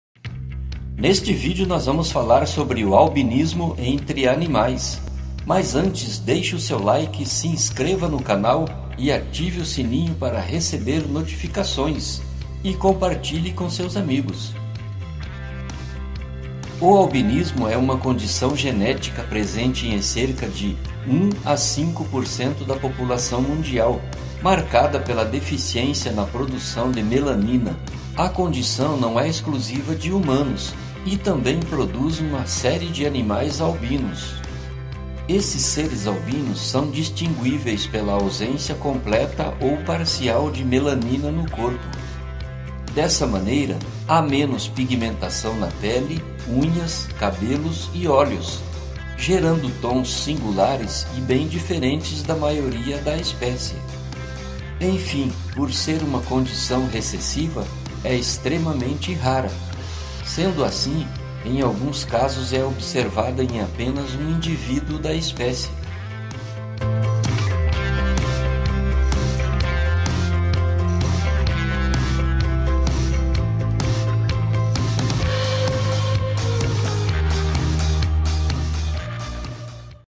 Animais Albinos - Criaturas Que Chamam Atenção Pela Cor - Tão Lindos Quantos os Coloridos.
Seja bem vindo (a) ao canal NaTuReZa ViVa!